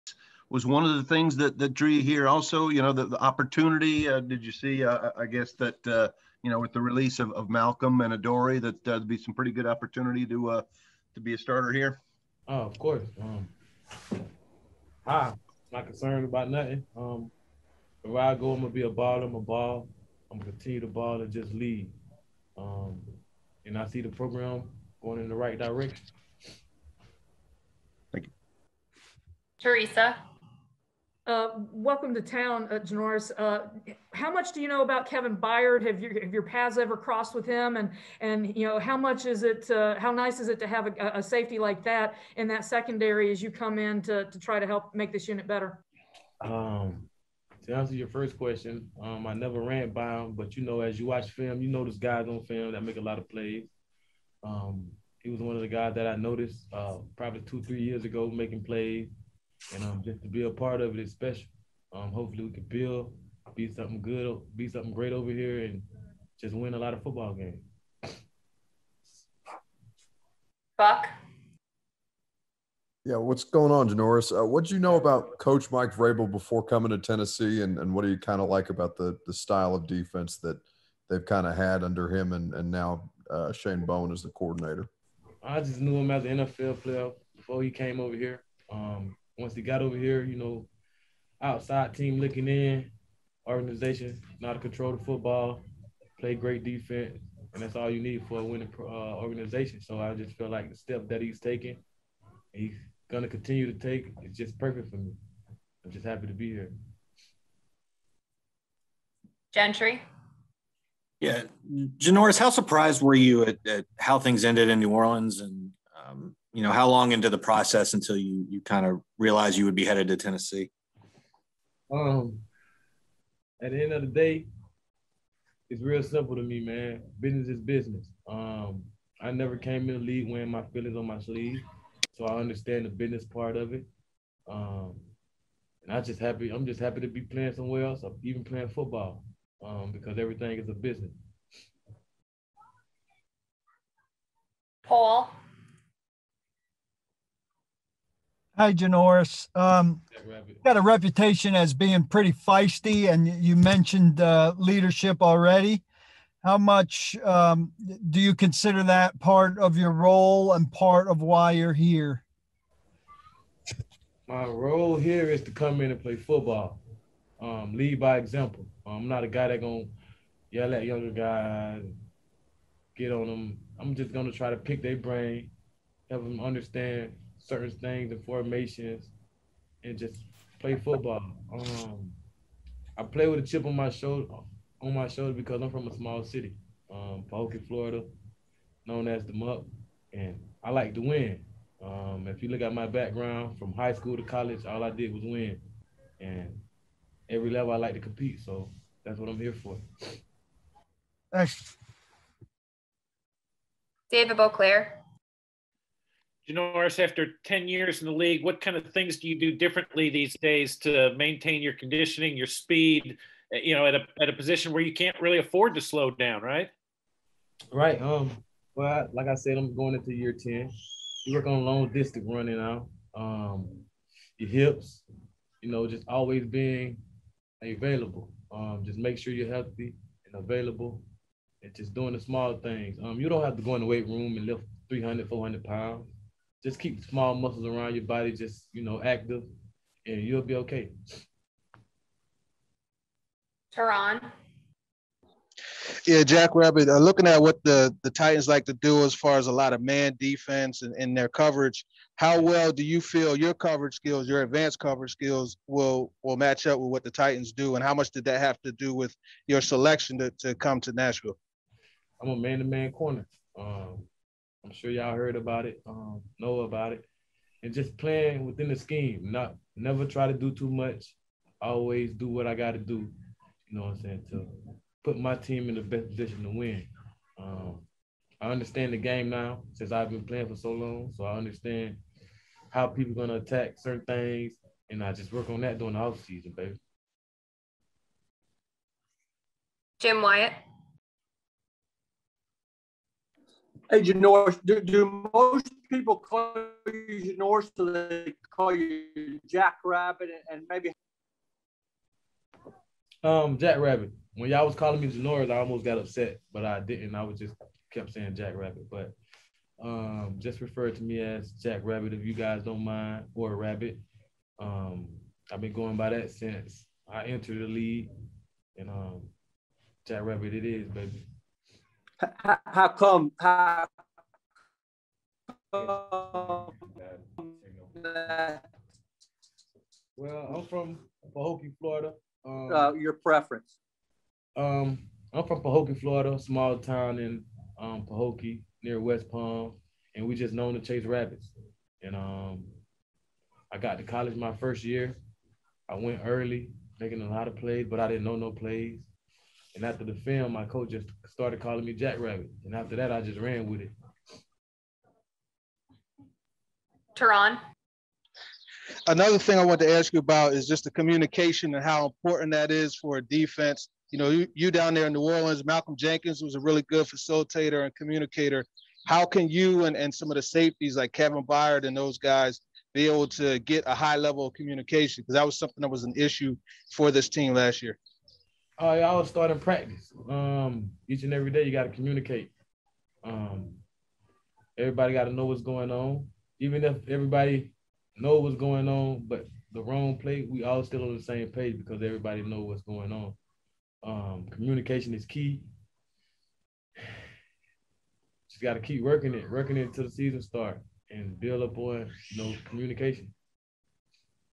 Janoris Jenkins: I Always Do What I Gotta Do
CB Janoris Jenkins speaks with reporters on Friday after signing with the Tennessee Titans.